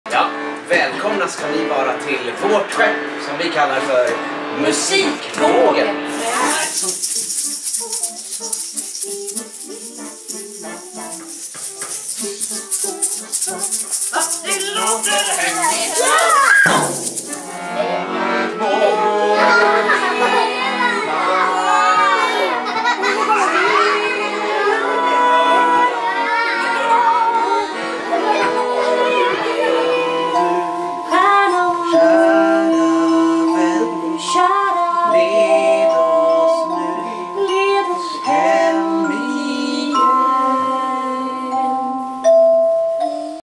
M/S Musikvågen
En musikalisk sagostund på böljan den blå.
Musik, rörelse och drama för både stora och små!
Kom och var med på detta härliga familjeäventyr! Skeppet M/S Musikvågen tar dig ut på en färd fylld med sånger, dans och lek i sagornas värld. I denna kurs får du tillsammans med ditt barn utforska en mix av kulturskolans ämnen; musik, dans och teater. Aktiviteten vänder sig till barn (3-6 år) och föräldrar i norra Botkyrka.